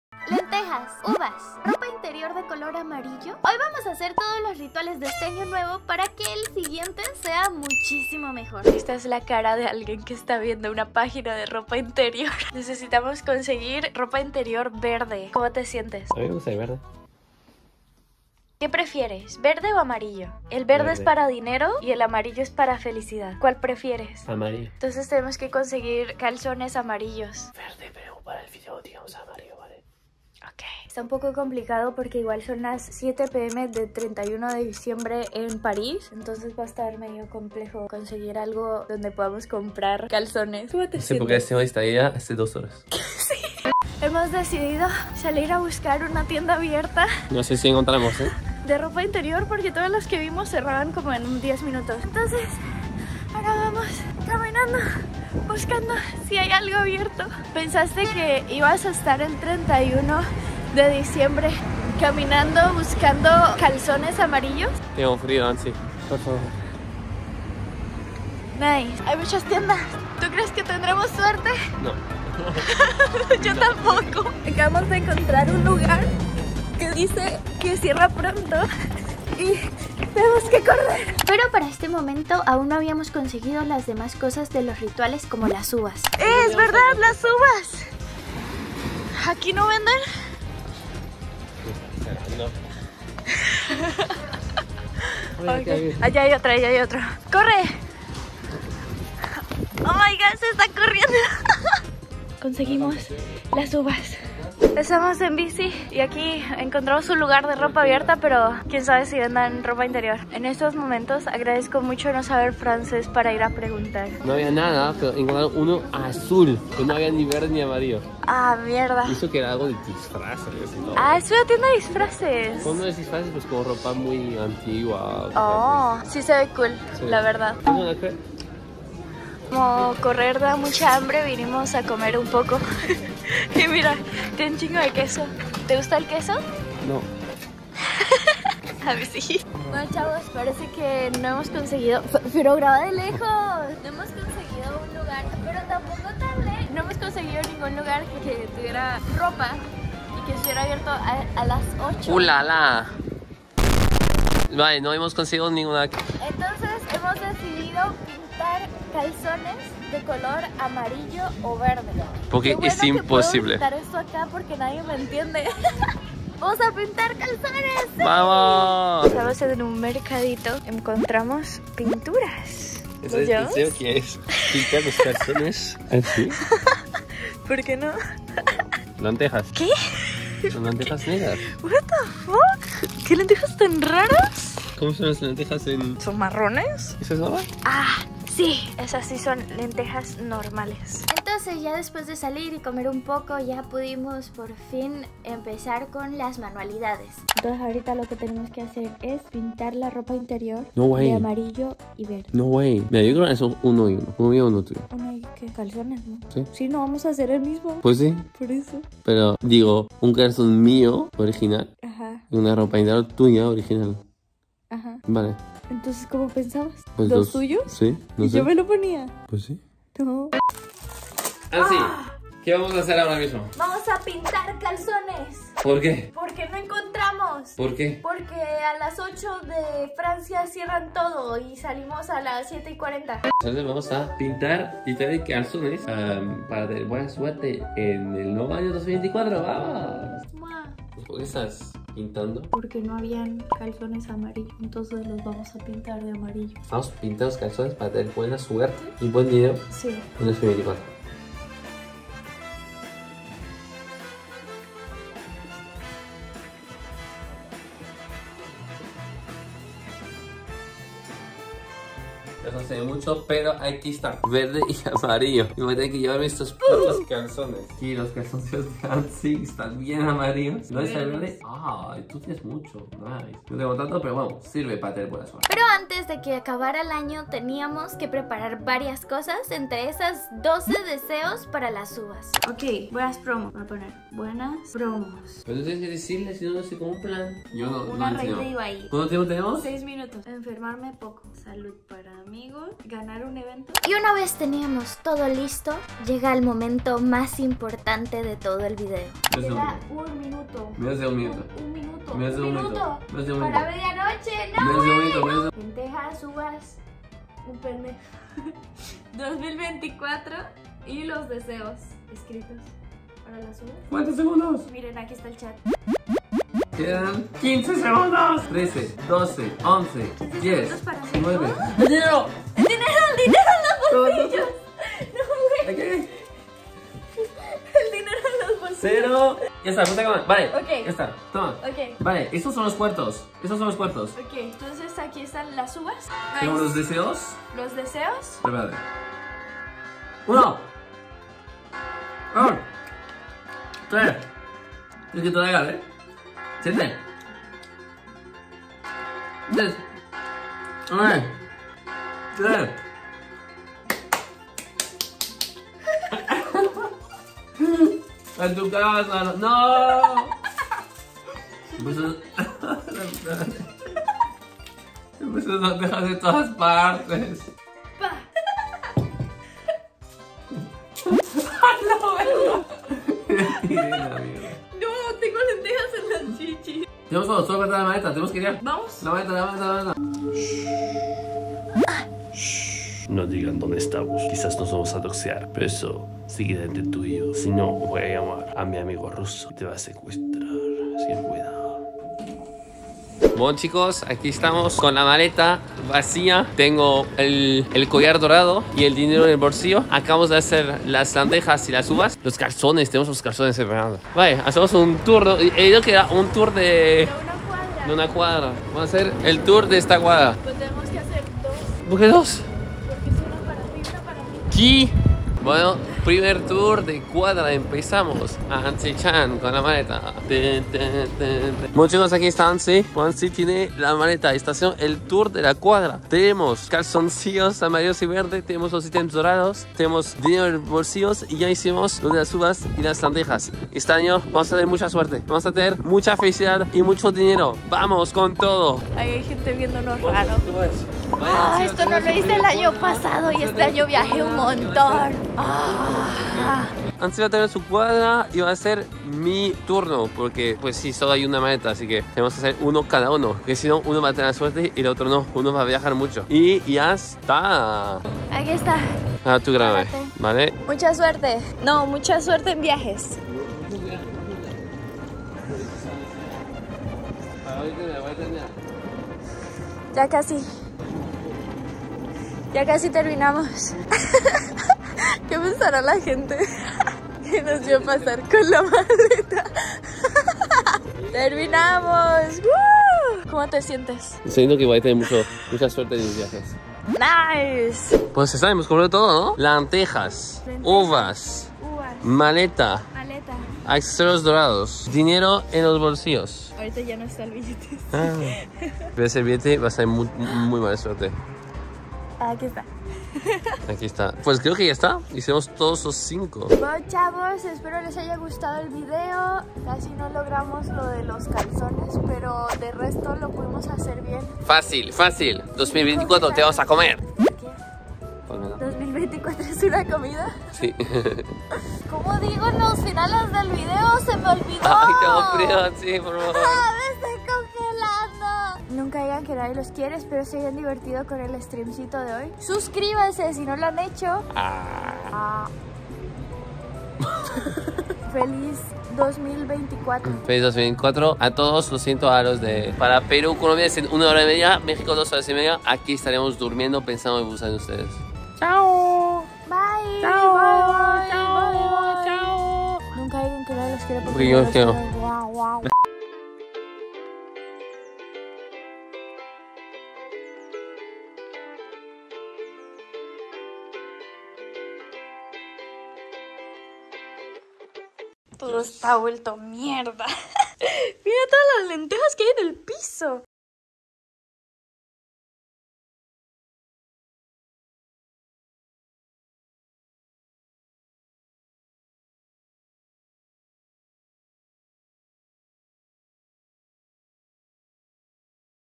Hice TODOS los rituales de FIN DE AÑO 🎉
Hoy haremos RITUALES para este AÑO NUEVO 2024, formas para tener una MEJOR SUERTE este año, junto a mi novela SHADOUNE